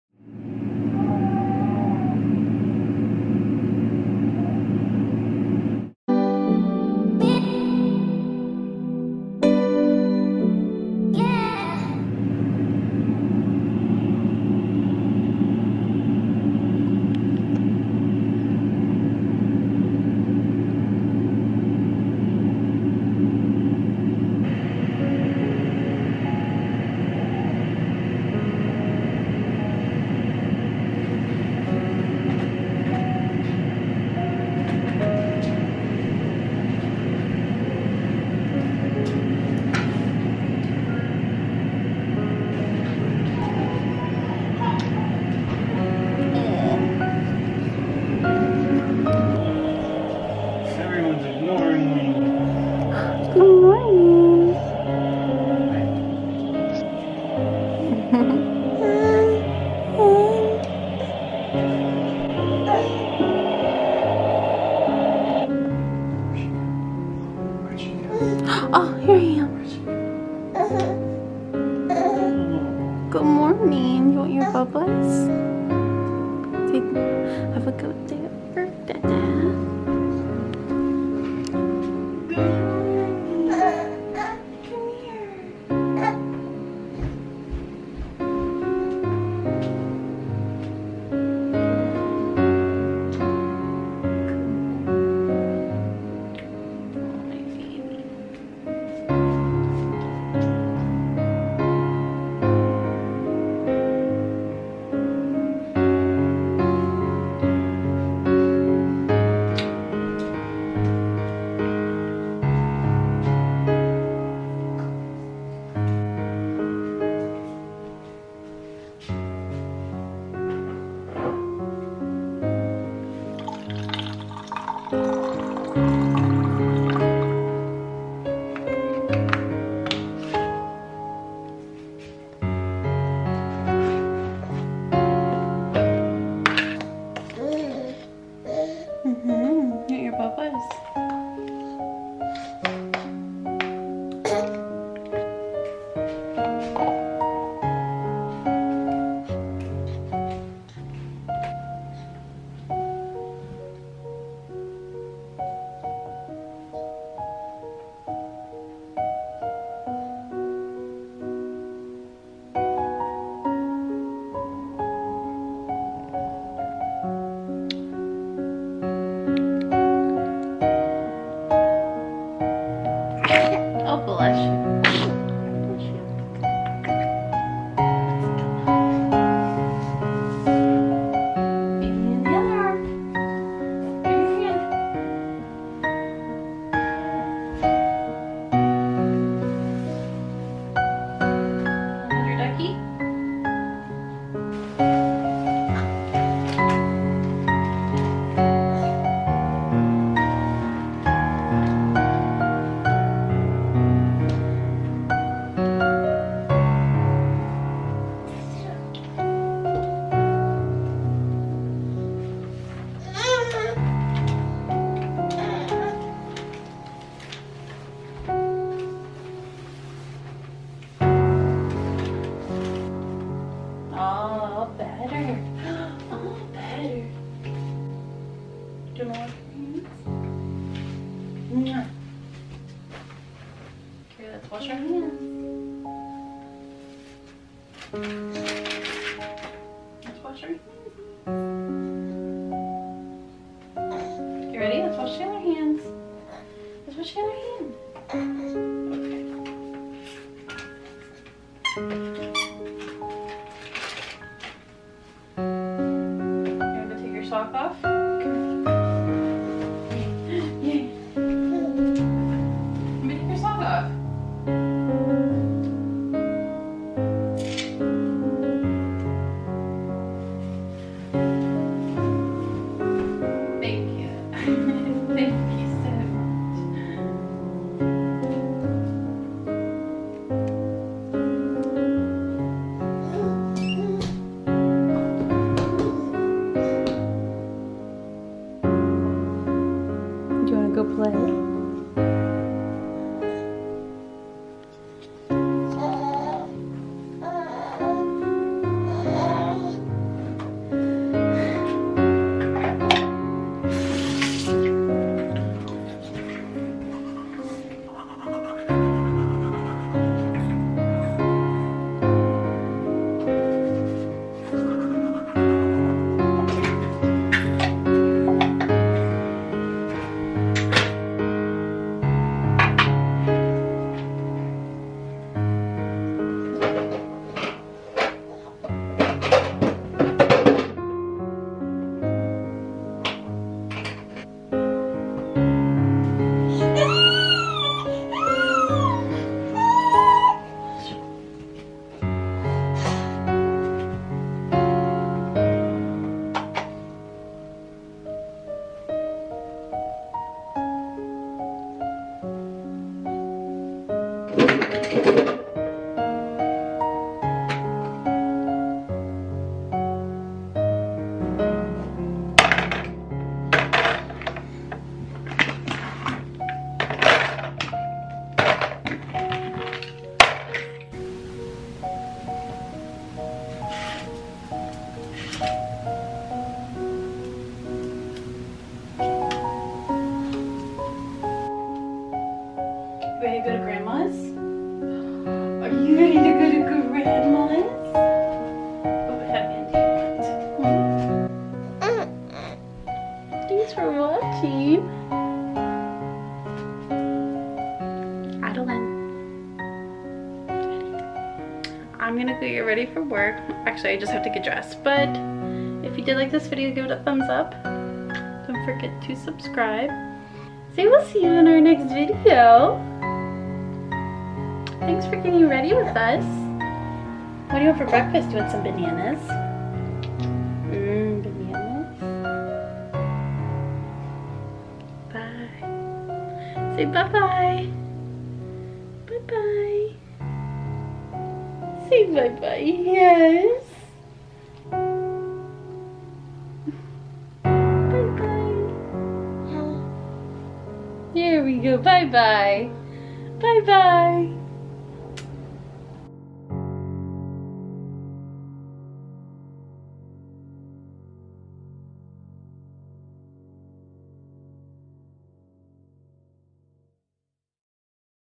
MOMMY MORNING ROUTINE | working mama
Good morning guys! Welcome to mine and Adalyn's morning routine for the days when I have to work.  (BRB CRYING) Enjoy!

I promise not allll the pictures I post are of Adalyn lol.